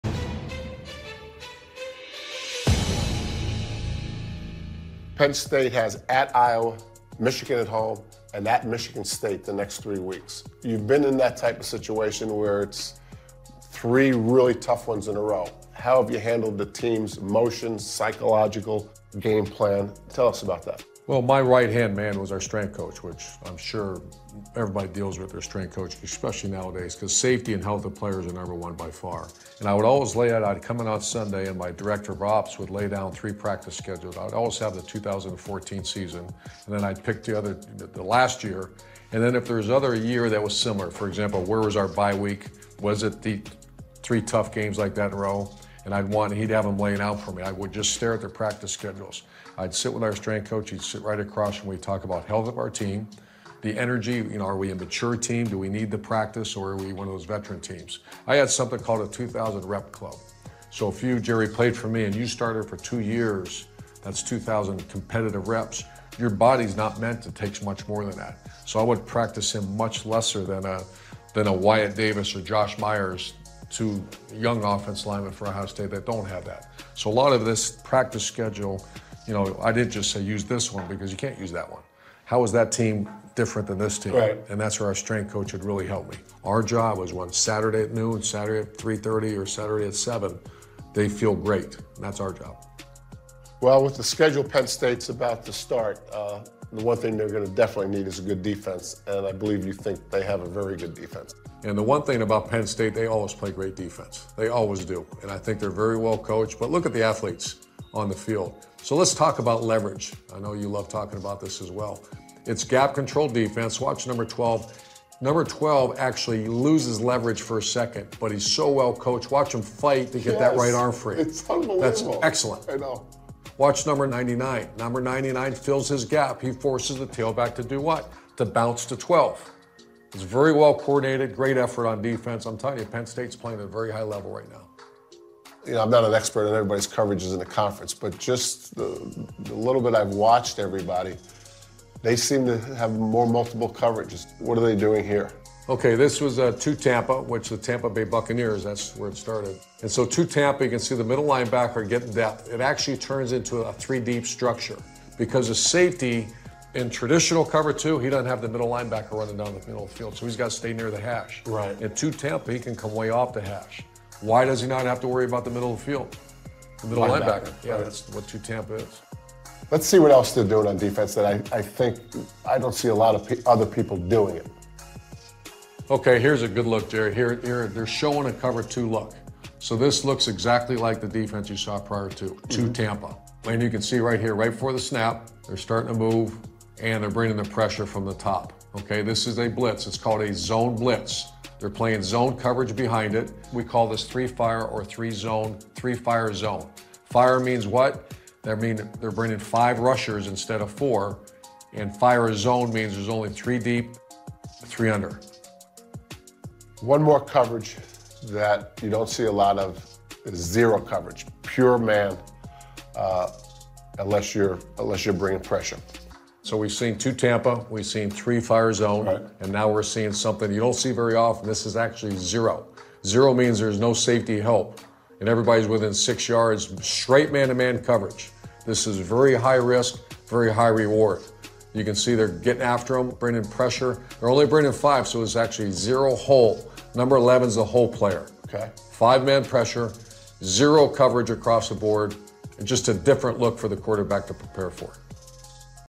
Urban Analysis: Breaking Down the Penn State Defense | B1G Football
Gerry DiNardo sits down with Urban Meyer for a close look at the Penn State defense.